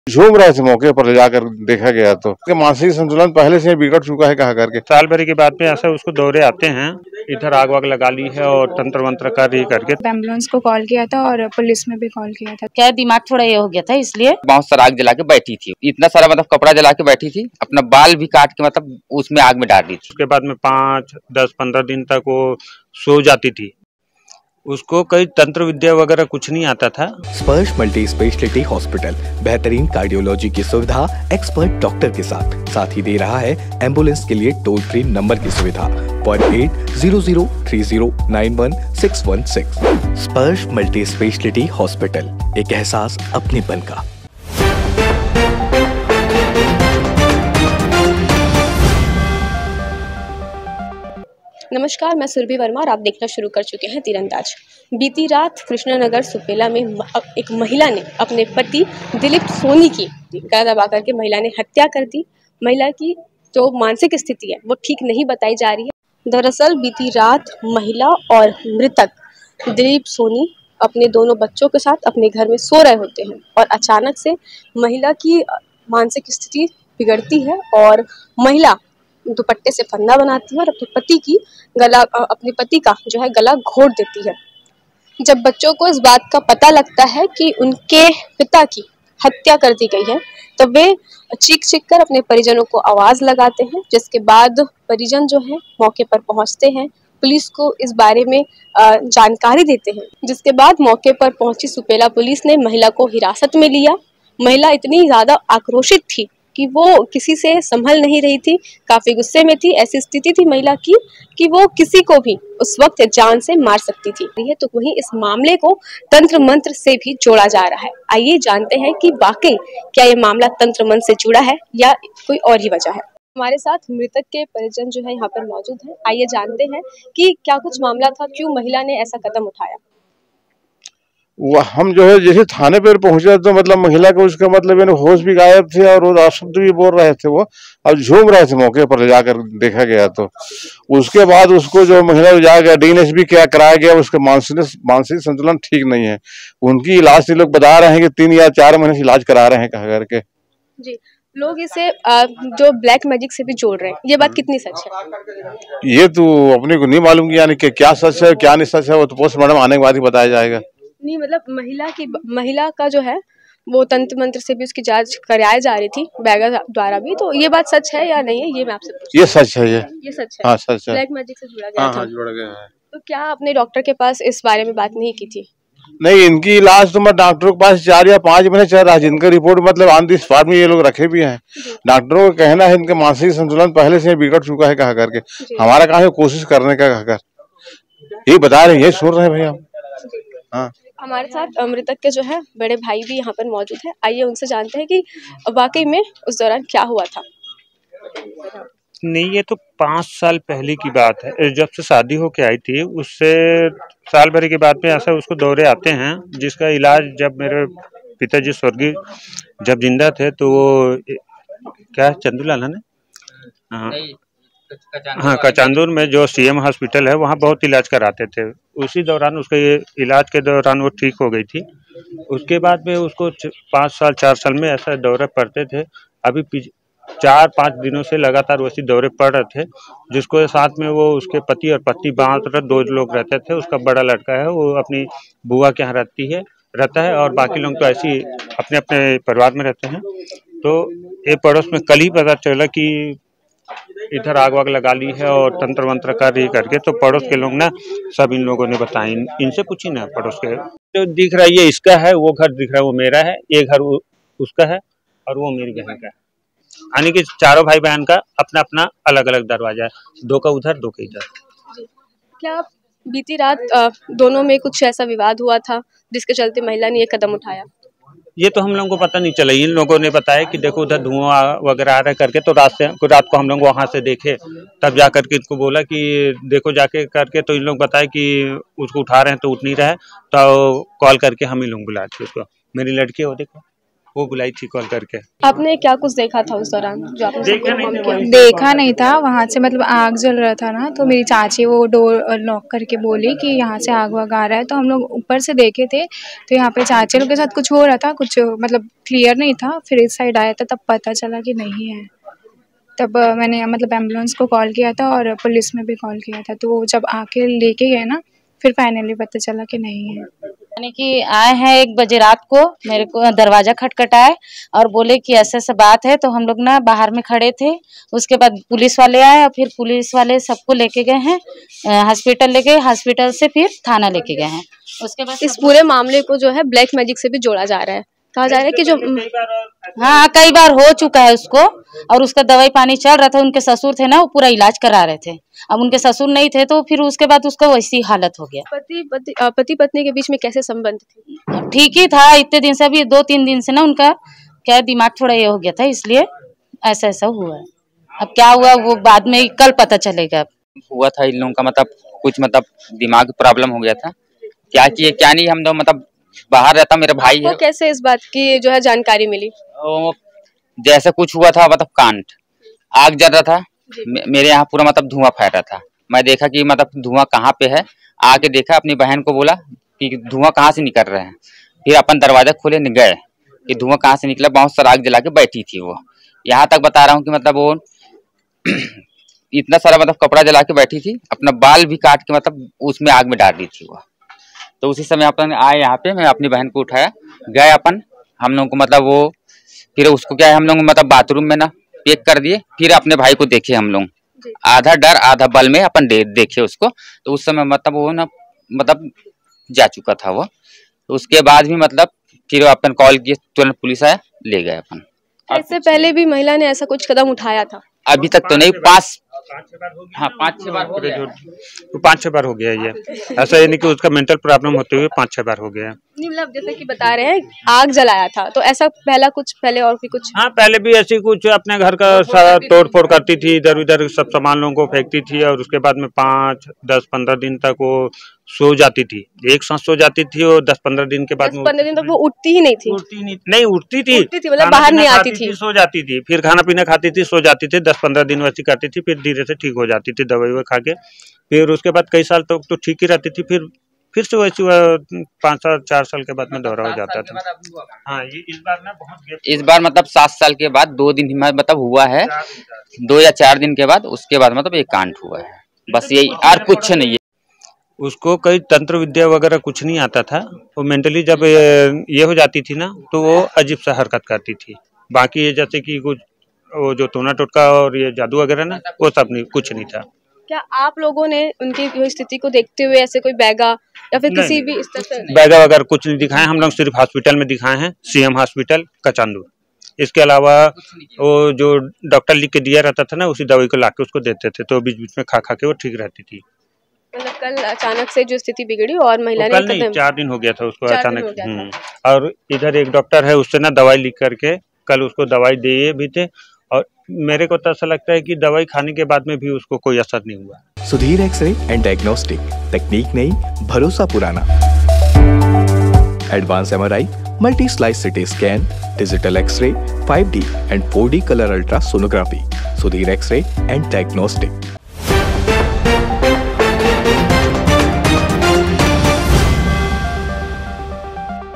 पति की जान की दुश्मनी बनी पत्नी | भिलाई में दिल दुखाने वाला कांड | Bhilai Crime News | Tirandaj
भिलाई में बीती रात एक बड़ा कांड हो गया। एक मानसिक रूप से बीमार महिला ने रात में सोते समय पति की जान ले ली। जब परिजन पहुंचे तो महिला कमरे में आग लगाकर कपड़े जलाती दिखी। महिला ने अपने बाल भी काटकर आग में जला दिए।
A big incident took place in Bhilai last night. A mentally ill woman killed her husband in his sleep at night. When the relatives reached, the woman was seen burning clothes by setting fire in the room. The woman also cut her hair and burnt it in the fire.

ताजा खबरों से अपडेट रहने डाउनलोड करें tirandaj का app